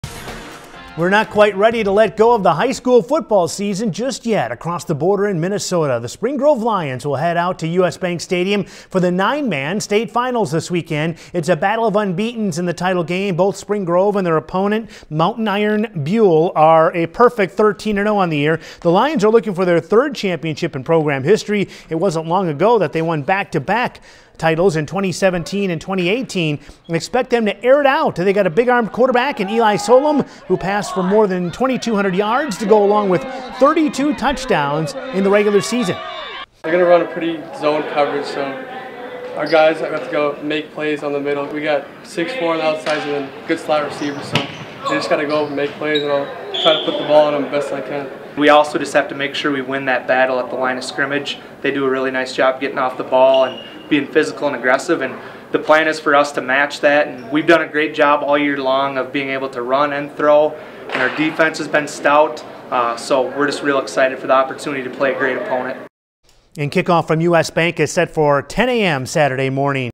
Spring Grove Lions hungry for third state title in school history
Spring Grove has a chance this weekend to further establish themselves as a powerhouse program in 9-man football in Minnesota. If they can beat Mountain Iron-Buhl, they'll take home their 3rd state title in the last six years.